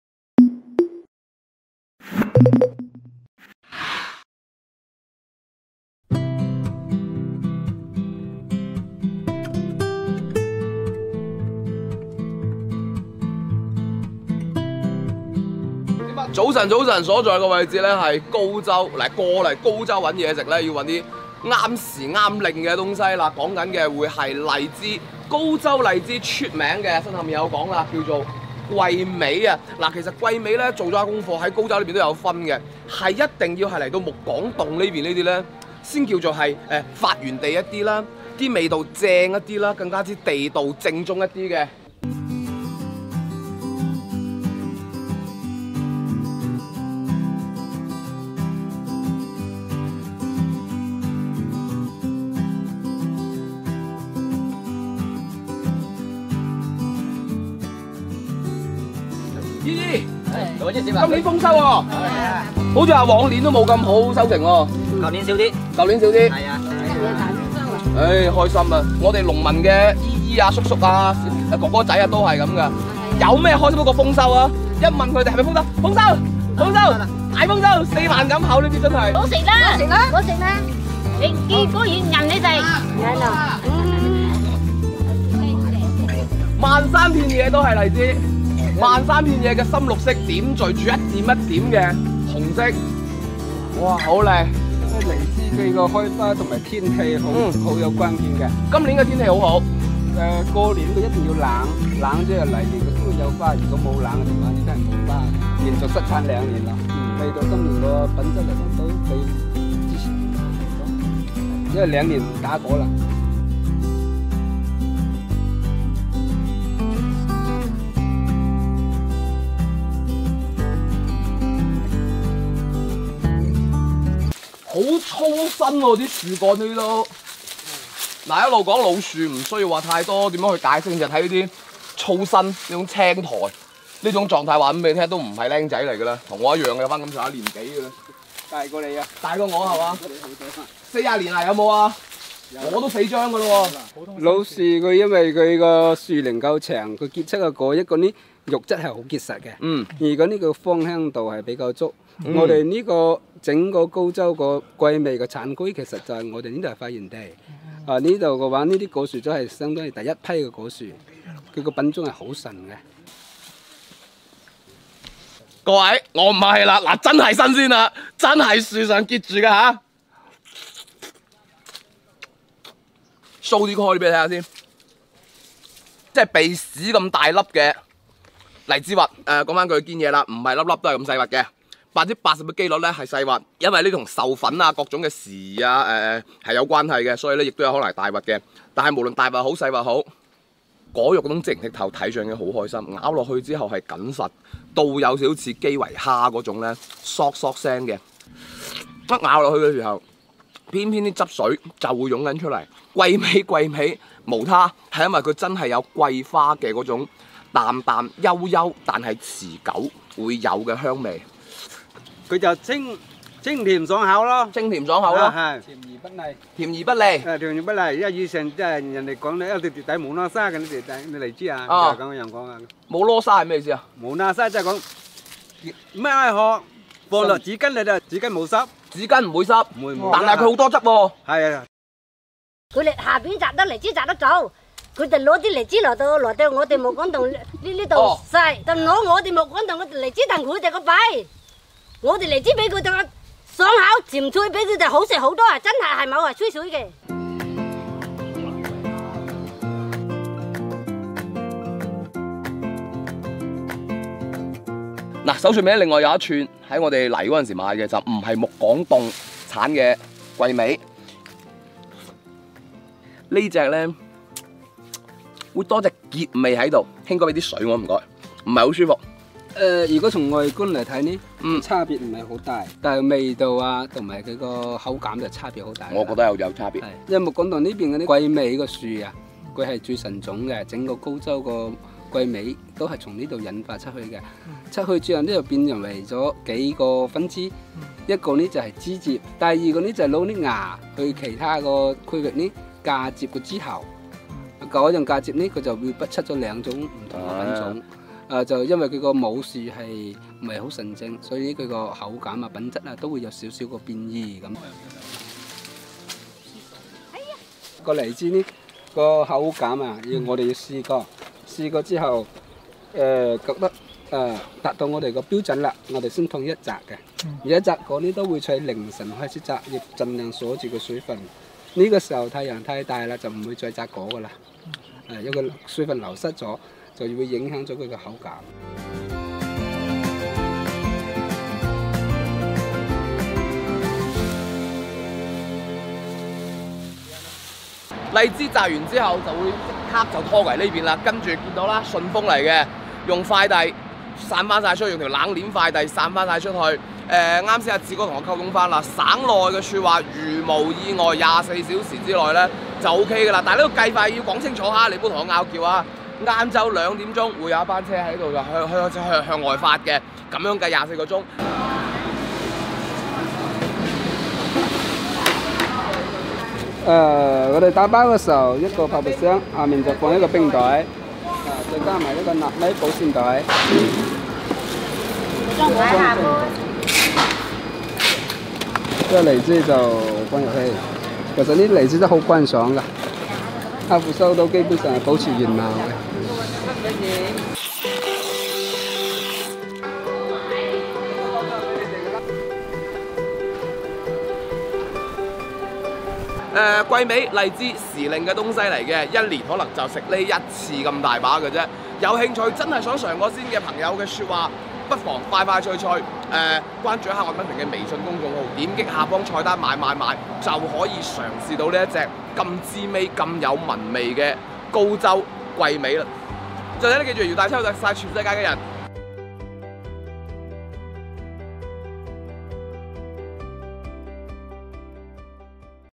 荔枝到處有得吃，但核心產地的荔枝，就沒那麼容易吃得到了！ /高州荔枝【品城記】
荔枝到處有得吃，但核心產地的荔枝，就沒那麼容易吃得到了！ /高州荔枝
今年荔枝收成不錯，趕緊來“桂味荔枝第一鄉”大飽口福吧！
進入6月，廣東各地的水果又相繼進入了成熟期，而所有水果當中，最令人期待的，毫無疑問就是荔枝了！當年，蘇東坡流放嶺南的時候，就曾經寫下過“日啖荔枝三百顆，不辭長作嶺南人”的名句，嶺南荔枝，能得到中國古代最有名的吃貨蘇東坡的點贊，足以證明它的口味超群！其實，廣東的荔枝根據地域、成熟時間的差異，又分為不同的品種，而【桂味荔枝】是公認品質最佳、口味最好的！上週，我們就親自去了一趟“桂味荔枝第一鄉”——高州的木廣垌，想要領略一下，聞名全國的【木廣垌桂味】究竟有多好吃！
吃遍广东
吃鸡！
品城記 探店 廣東 荔枝 高州荔枝 高州 "我們將確保影片內容不涉及任何動物虐待行為，所有食材的取得都符合道德和法律規定。"